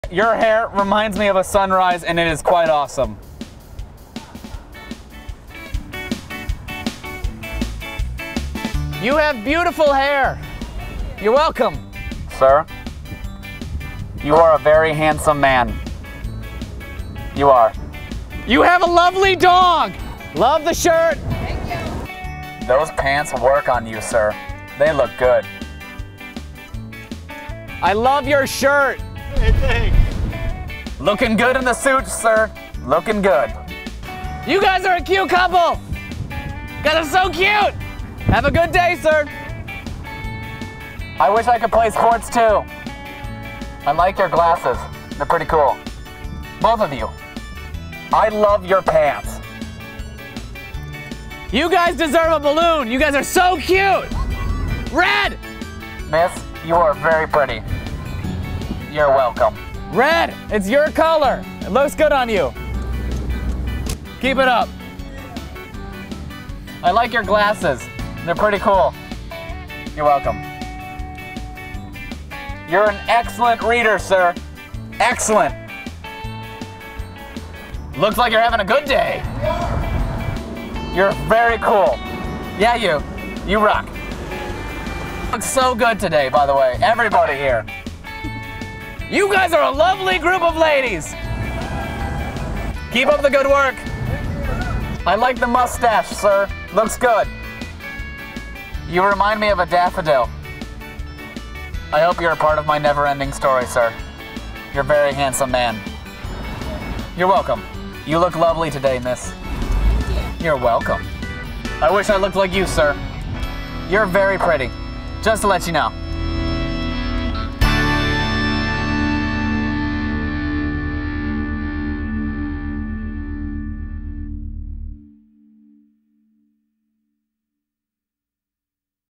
Drive By Compliments 2
We jumped in a car and yelled nice things to people... We even used a megaphone!

This video has been seen on Mashable, Huffington Post, ABC News, Yahoo News and a random website in Croatian (we assume it says good things.)